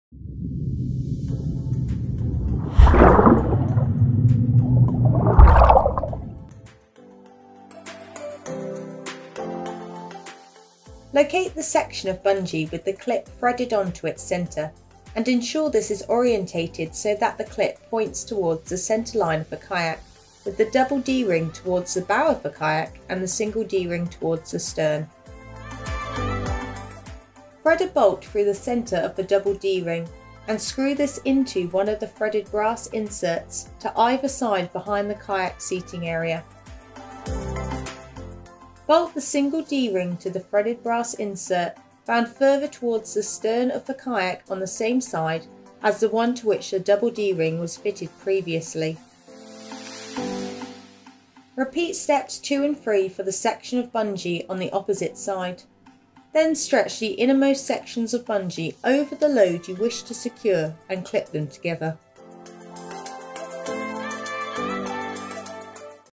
SurfJet 2.0 Fitting Guide: Storage Area Bungees
How to install the Storage Area Bungees Kit (MS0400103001) on your Pyranha SurfJet 2.0:

Tools Required: 5mm Allen Key

Optional: You may wish to add a small amount of Threadlock to each bolt to prevent them coming loose during usage and transport.

Step 1: Locate the section of bungee with the clip threaded onto its centre, and ensure this is orientated so that the clip points towards the centreline of the kayak, with the double D-Ring towards the bow of the kayak and the single D-ring towards the stern.

Note: If you already have the Surf Zone Backrest Kit installed, you will need to remove the single D-rings behind the kayak’s seating area and rethread the backrest bungees through the free side of the double D-rings supplied as part of the Storage Area Bungees Kit once installed.

Step 2: Thread a bolt through the centre of the double D-ring and screw this into one of the threaded brass inserts to either side behind the kayak’s seating area.

Step 3: Bolt the single D-ring to the threaded brass insert found further towards the stern of the kayak on the same side as the one to which the double D-ring was fitted previously.

Step 4: Repeat Steps 2 and 3 for the section of bungee on the opposite side, then stretch the innermost sections of bungee over the load you wish to secure and clip them together.

🎥: Tom Clare Photography